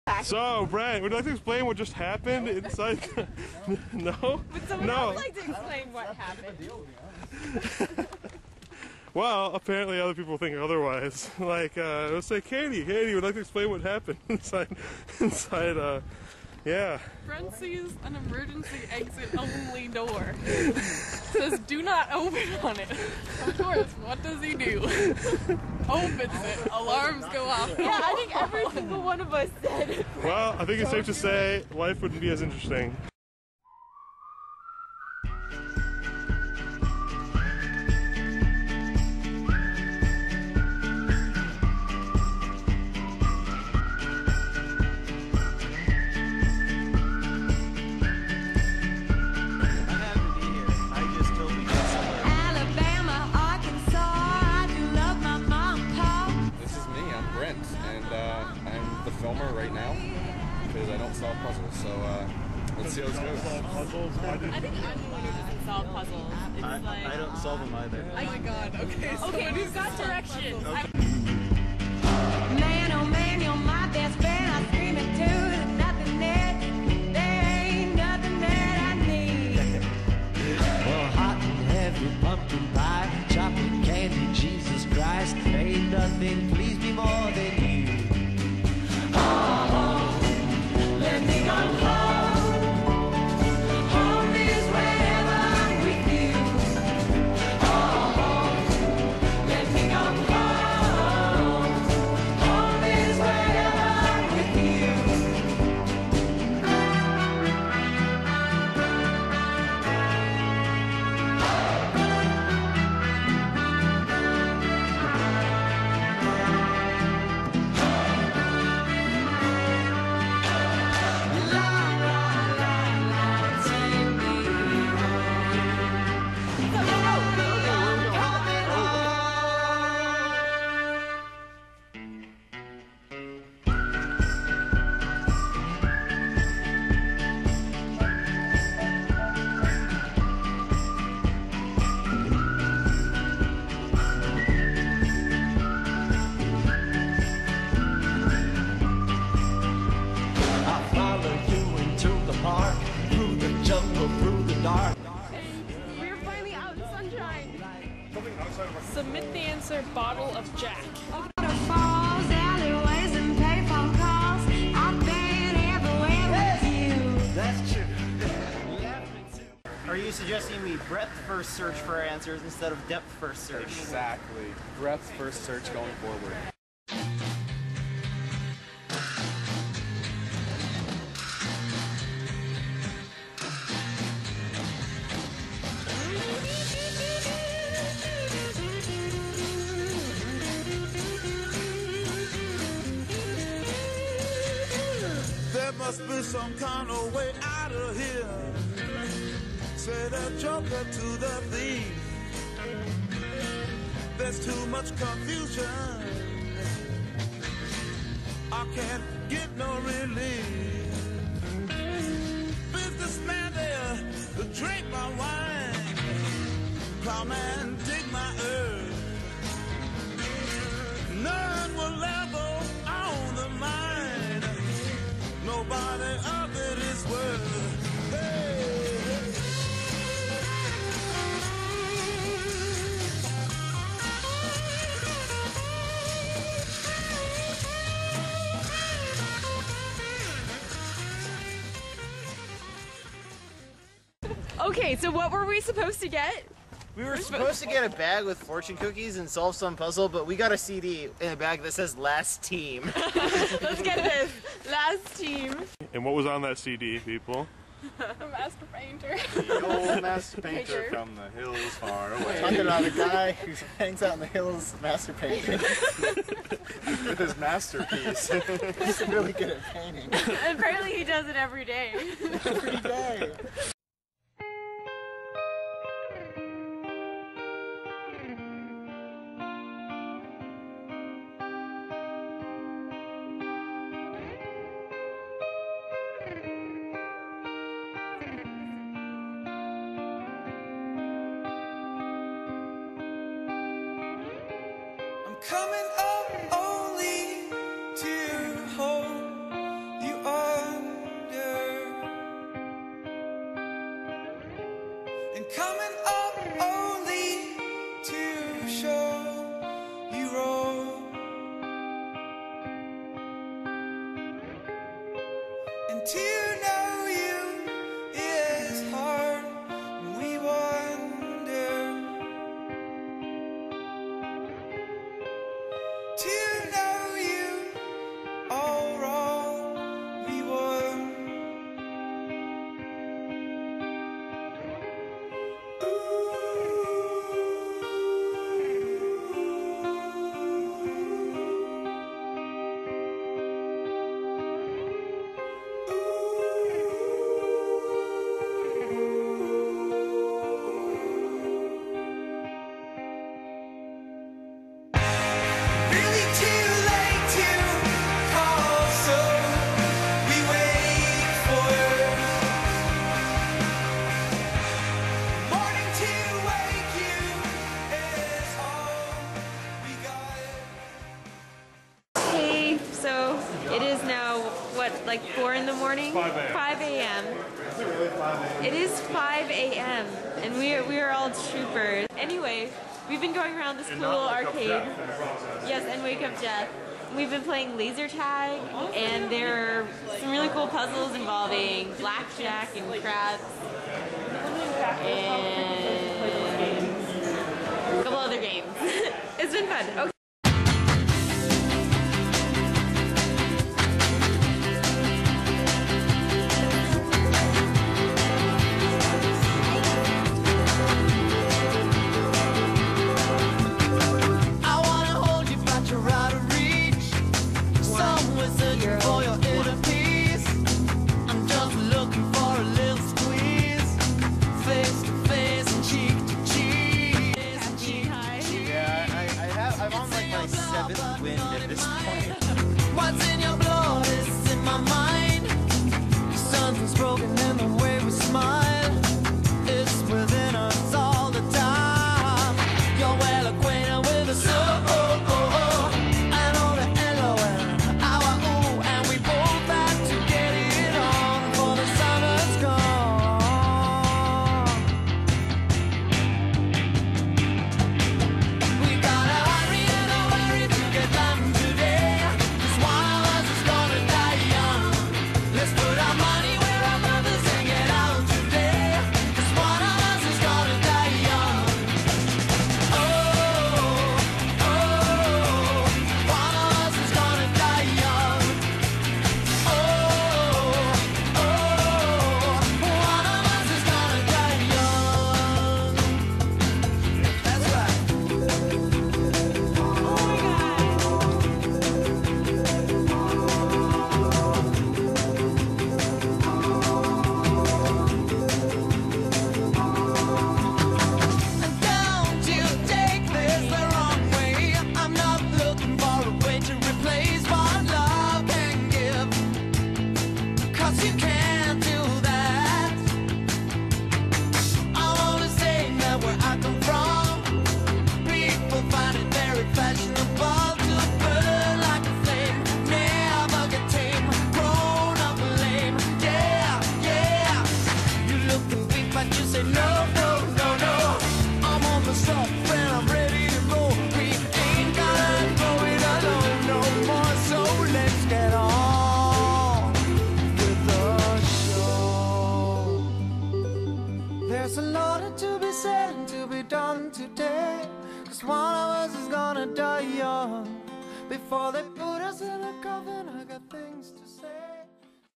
Intern Game 2010 - Our Documentary
Six interns at Microsoft spend 36 hours driving all around Washington state solving puzzles in an event called Intern Game. We go to Seattle, the capitol, Jimi Hendrix's grave, Microsoft HQ and more. Enjoy.